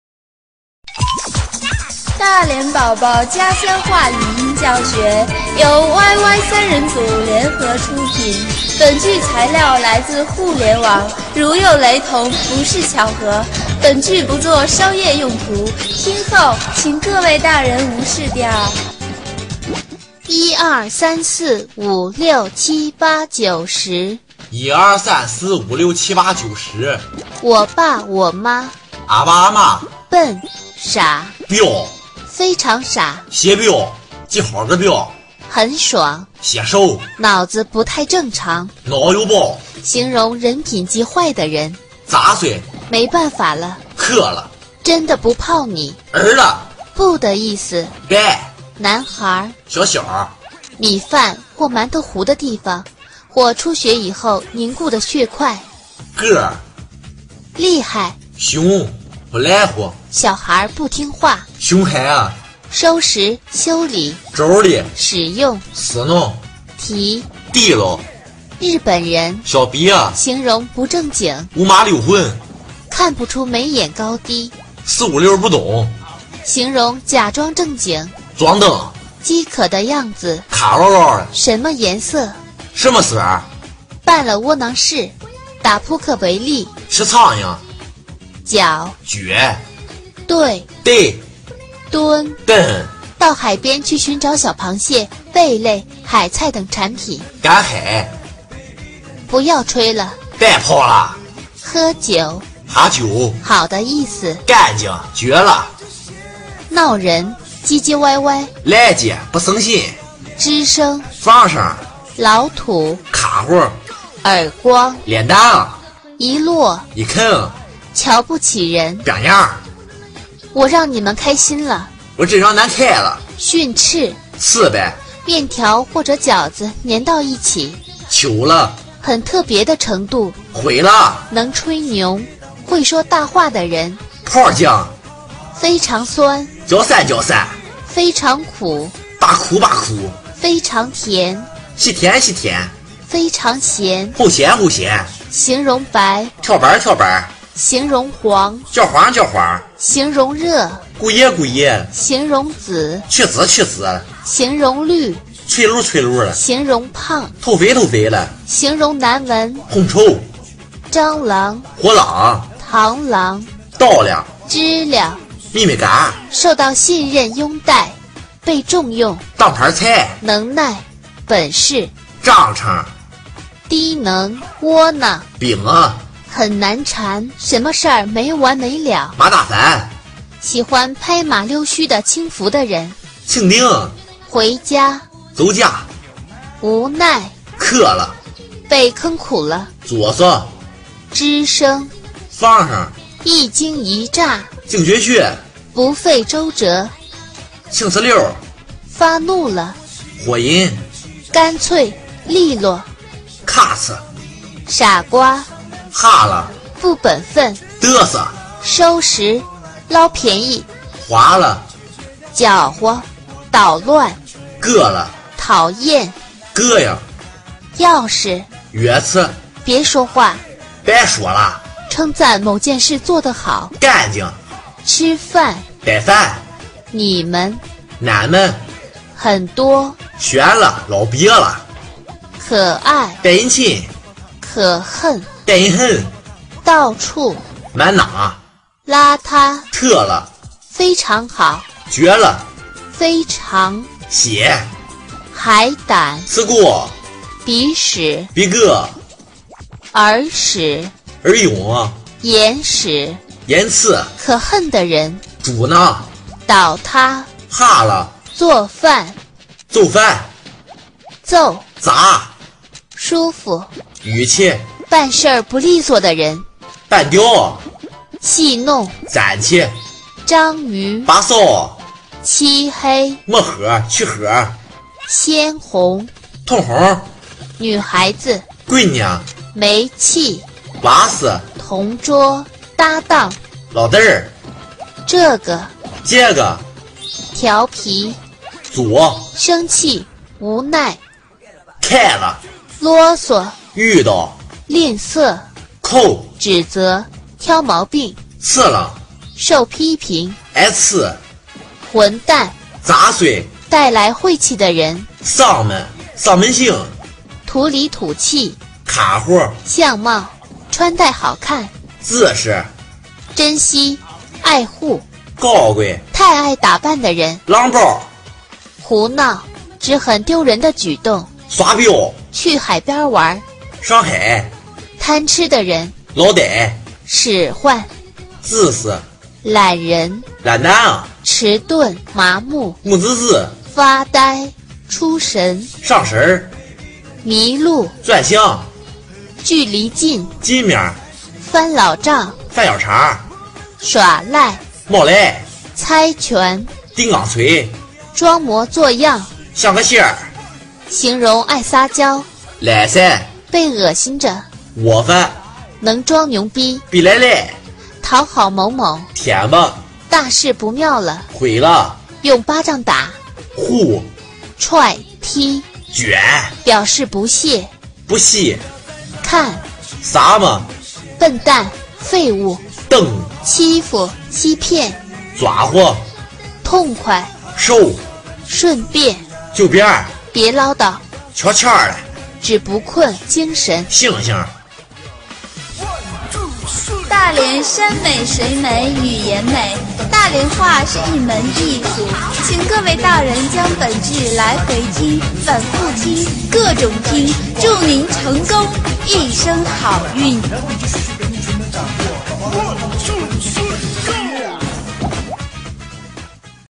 绝对正宗大连话
大连人说大连话！这是一段经典的大连话逐词解释的视频。敬请欣赏！